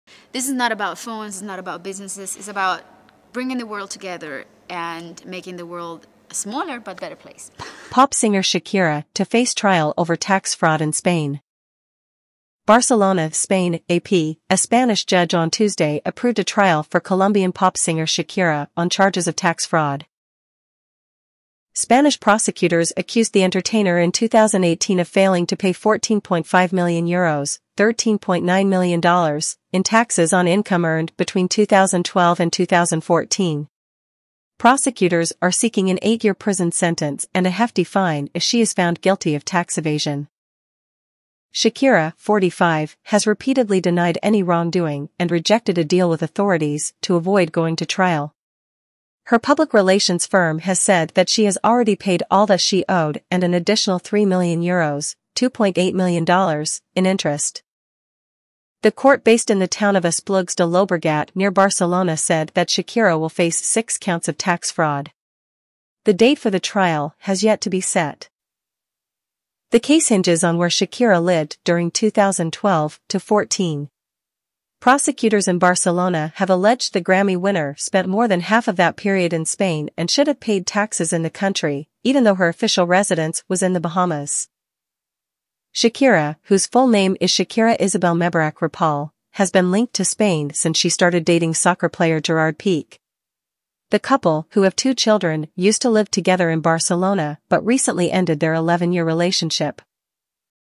Shakira to face trial over tax fraud in Spain
Spanish judge on Tuesday approved a trial for Colombian pop singer Shakira on charges of tax fraud.

Spanish prosecutors accused the entertainer in 2018 of failing to pay 14.5 million euros ($13.9 million) in taxes on income earned between 2012 and 2014. Prosecutors are seeking an eight-year prison sentence and a hefty fine if she is found guilty of tax evasion.

Shakira, 45, has repeatedly denied any wrongdoing and rejected a deal with authorities to avoid going to trial. Her public relations firm has said that she has already paid all that she owed and an additional 3 million euros ($2.8 million) in interest.

The court based in the town of Esplugues de Llobregat near Barcelona said that Shakira will face six counts of tax fraud. The date for the trial has yet to be set.

The case hinges on where Shakira lived during 2012-14. Prosecutors in Barcelona have alleged the Grammy winner spent more than half of that period in Spain and should have paid taxes in the country, even though her official residence was in the Bahamas.

Shakira, whose full name is Shakira Isabel Mebarak Ripoll, has been linked to Spain since she started dating soccer player Gerard Pique. The couple, who have two children, used to live together in Barcelona but recently ended their 11-year relationship.

Spain has cracked down on soccer stars like Lionel Messi and Cristiano Ronaldo over the past decade for not paying their full due in taxes. They were found guilty of evasion but both avoided prison time thanks to a provision that allows a judge to waive sentences under two years in length for first-time offenders.